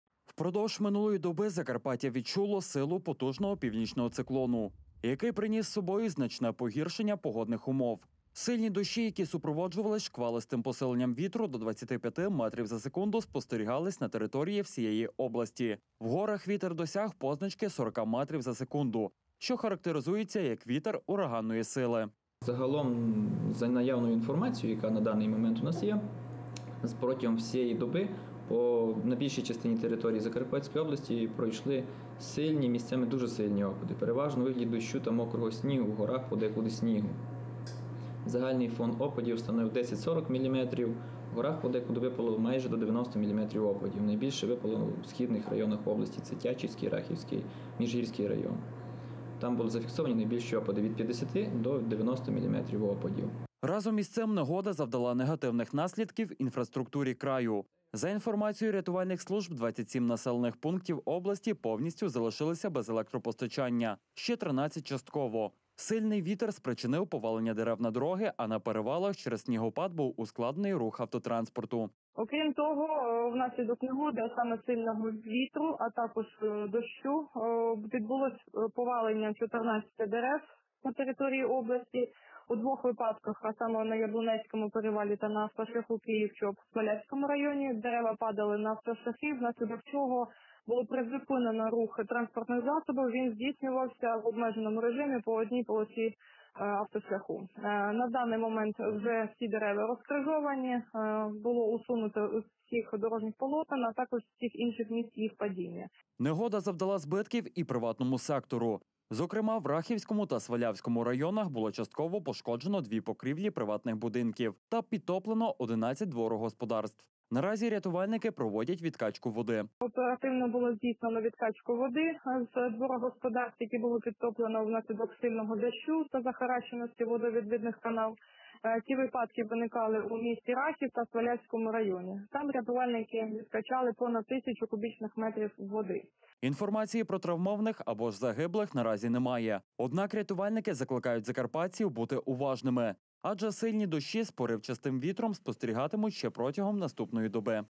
Наслідки негоди
Впродовж минулої доби Закарпаття відчуло силу потужного північного циклону, який приніс з собою значне погіршення погодних умов. Сильні дощі, які супроводжувались шквалистим посиленням вітру до 25 метрів за секунду, спостерігались на території всієї області. В горах вітер досягав позначки 40-ка метрів за секунду що характеризується як вітер ураганної сили. Разом із цим, негода завдала негативних наслідків інфраструктурі краю. За інформацією рятувальних служб, 27 населених пунктів області повністю залишились без електропостачання, ще 13 частково. Сильний вітер спричинив повалення дерев на дороги, а на перевалах, через снігопад, був ускладнений рух автотранспорту. Негода завдала збитків і приватному сектору. Зокрема у Рахівському та Свалявському районах було частково пошкоджено 2 покрівлі приватних будинків та підтоплено 11 дворогосподарств. Наразі рятувальники проводять відкачку води. Інформації про травмованих або ж загиблих наразі немає. Однак рятувальники закликають закарпатців бути уважними, адже сильні дощі з поривчастим вітром спостерігатимуть ще протягом наступної доби.